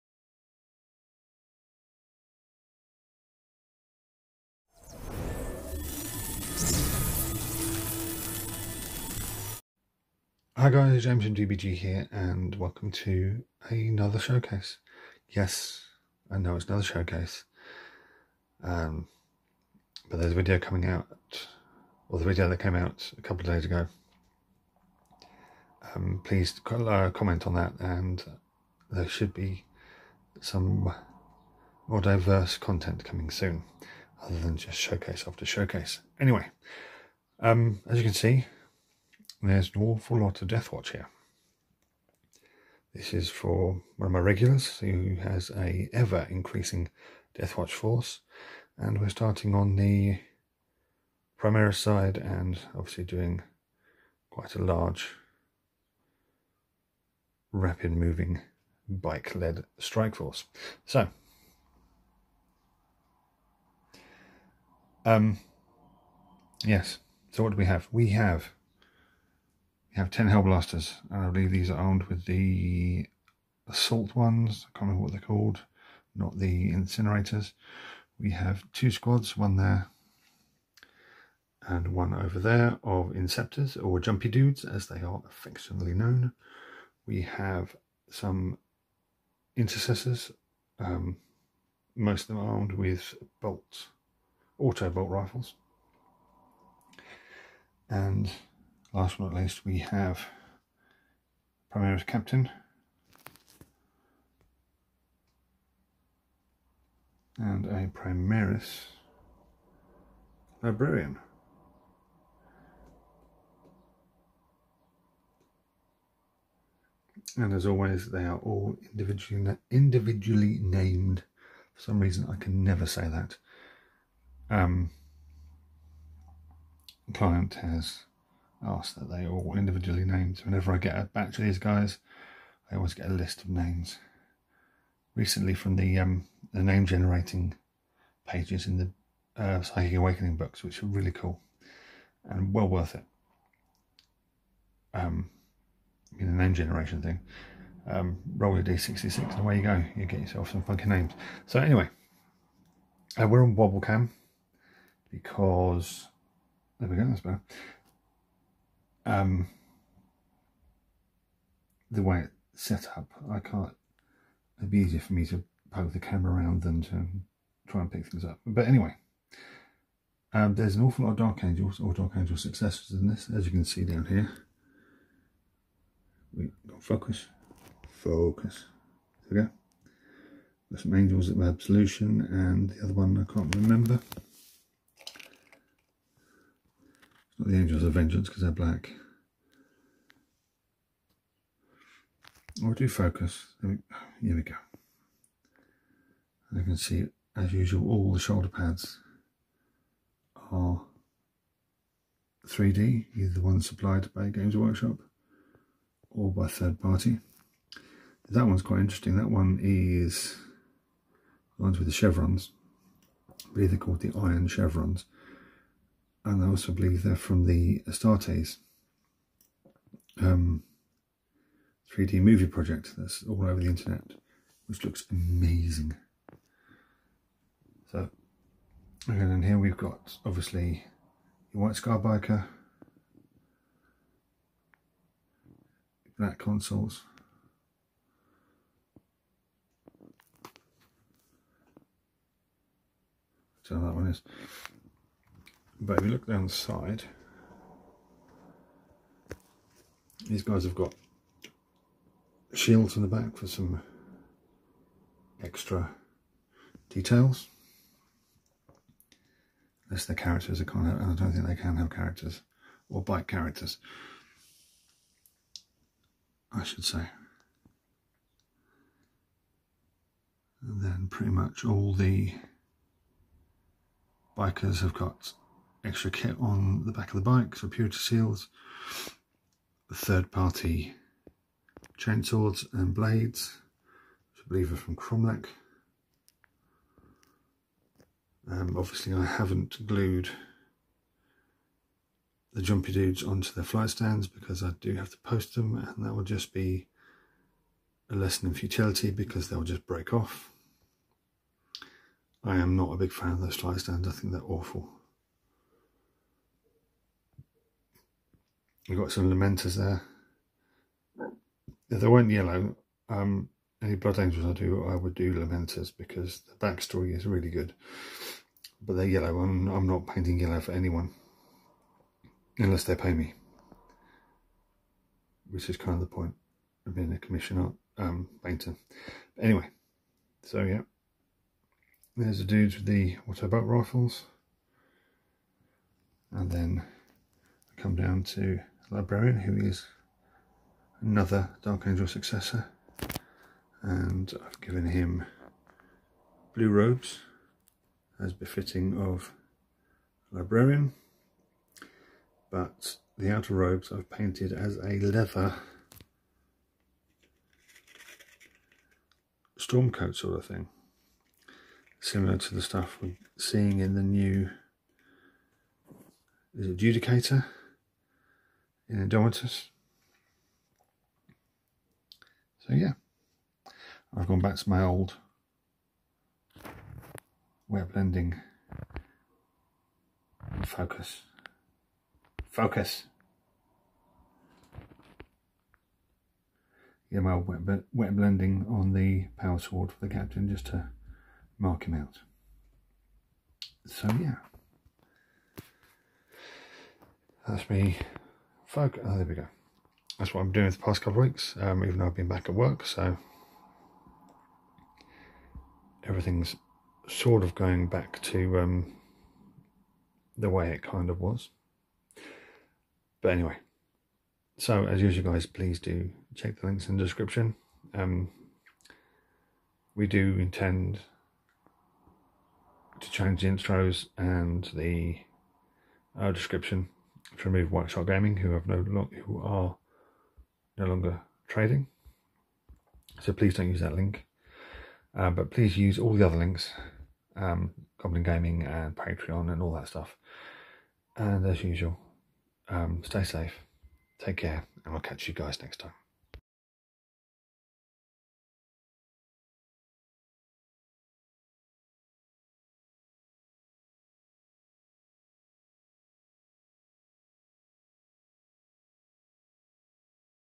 Death Watch Showcase
That's right another Showcase, this time of the continuing Death Watch commission. I've been doing for some time now. This time theres some Primaris units.